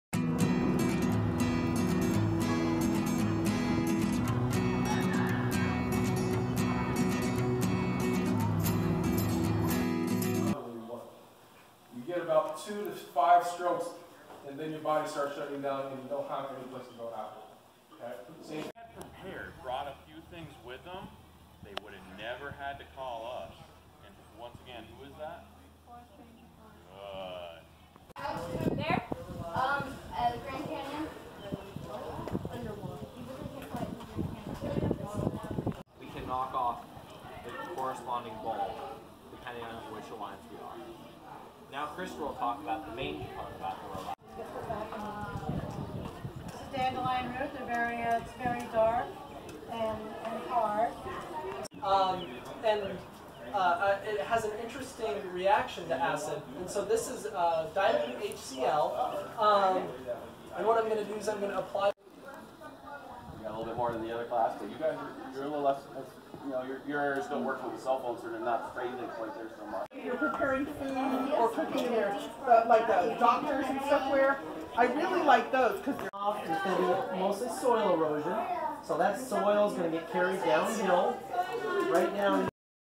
2018 Environmental Awareness Field Days
A sneak peak at our 2018 Environmental Awareness Field Days...over 400 students have attended with 10 organizations volunteering their time to educate the youth on the environment and being safe while out in the environment. We have everything from robotics, to beekeeping, geology to the NYS Troopers Marine Patrol and NYS FOREST Rangers.